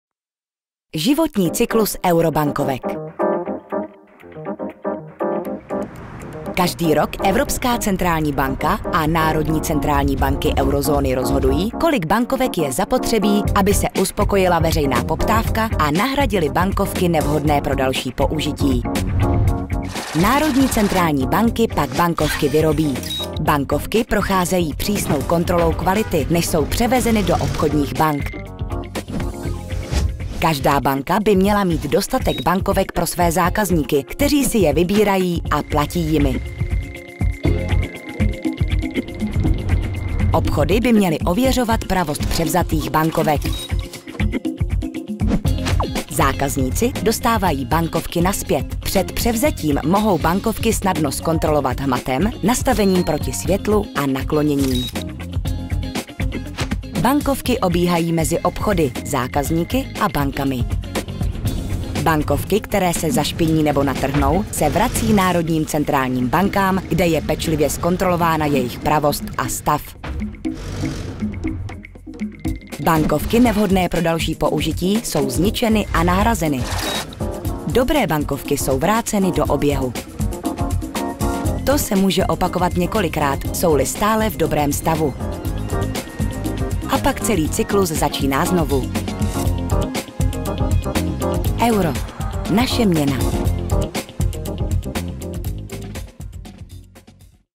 Životní cyklus eurobankovek
Kdo eurobankovky vyrábí? Jak dlouho jsou v oběhu? Najděte odpovědi v této krátké animaci o životním cyklu eurobankovek. Více informací o eurobankovkách je k dispozici zde: